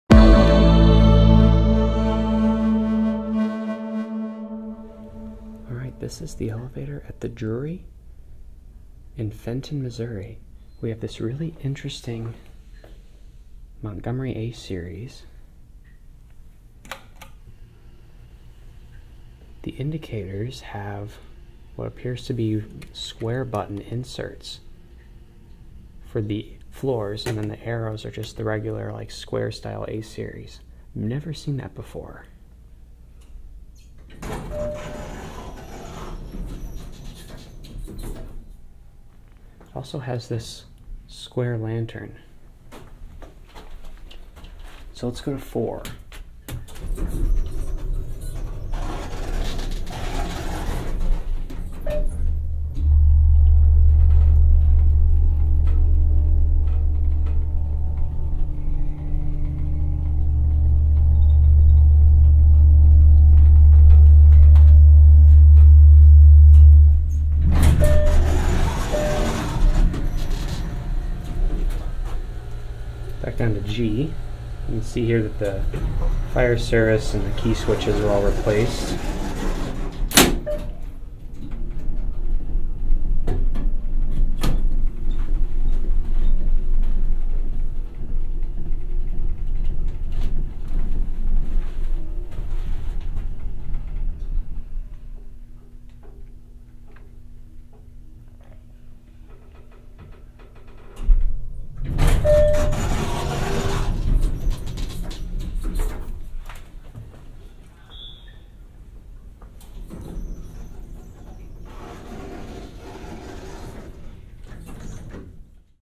Awesome Montgomery Hydraulic Elevator at the Drury in Fenton, MO
(February 2023) This is a really interesting Montgomery elevator at the Drury Inn in Fenton, MO. It has a really strange set of A series fixtures. Both of the floor indicators have old square buttons being used as the floor labels. The keyswitch panel was also replaced with various other keyswitches.